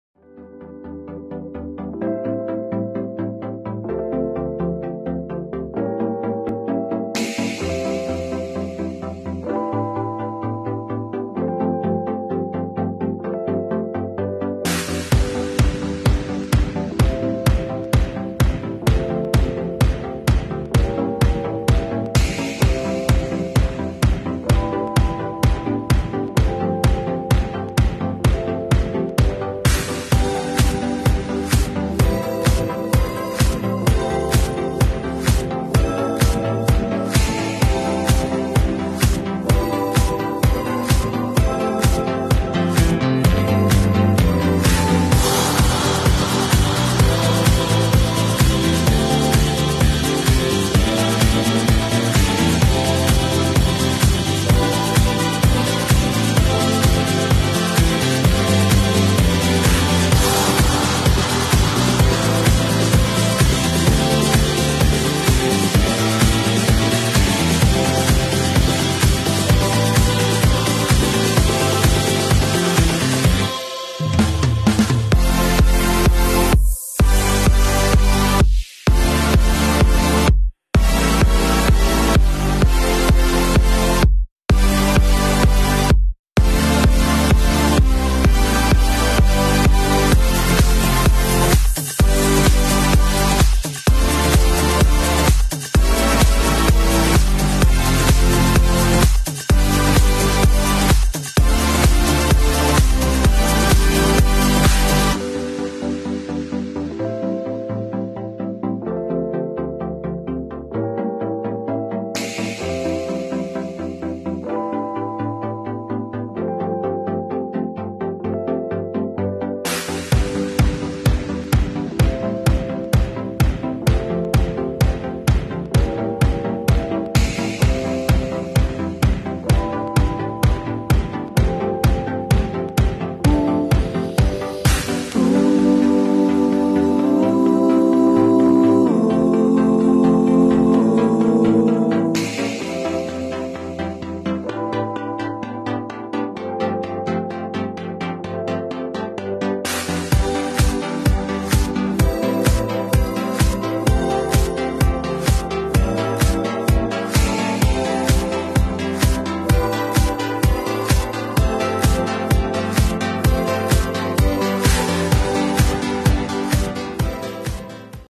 84 Rabbit GTI ( A second chance)
A glimpse of my GTI from before to what he is in thr present,  2018...